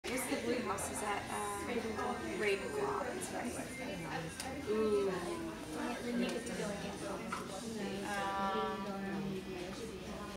Harry Potter birthday
Wednesday was Harry Potter's 39th birthday and celebrated at The Coffee Mugg(le) by his fans.